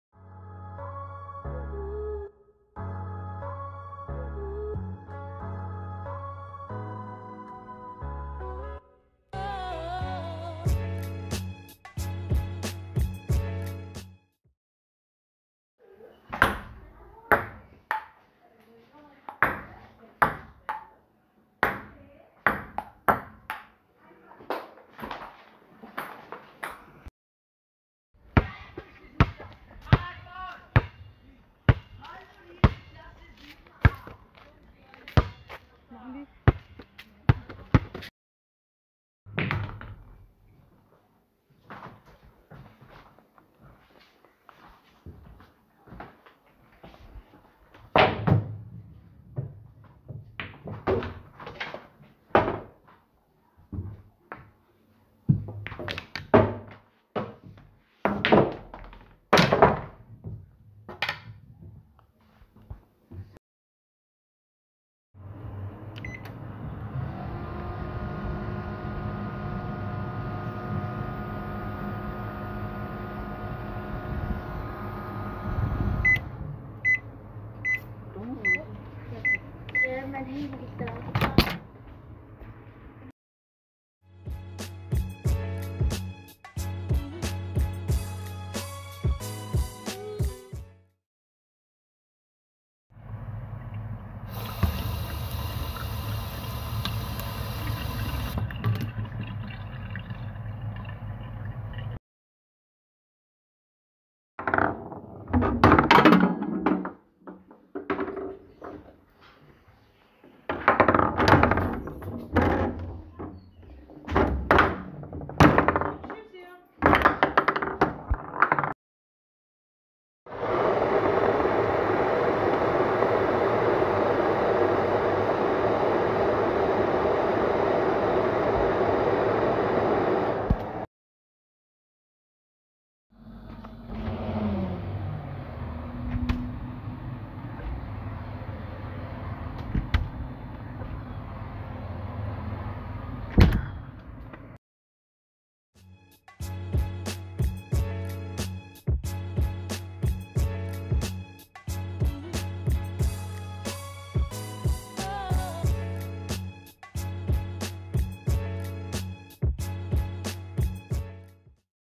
Hör - Challenge
Zusammen mit Kindern haben wir Mitarbeiter des Freizeittreffs Eckernfeld Geräusche, welche bei uns alltäglich sind, aufgenommen und als eine Hör-Challenge zusammen geschnitten.
Wie viele Geräusche könnt ihr erkennen?